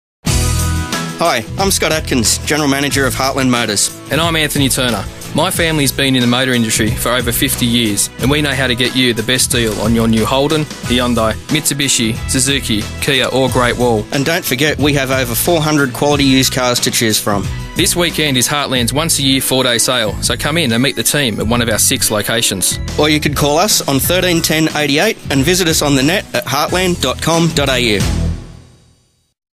2GB RADIO AD_0001.wmv
Heartland Motors 2GB radio ad for this weekends Sale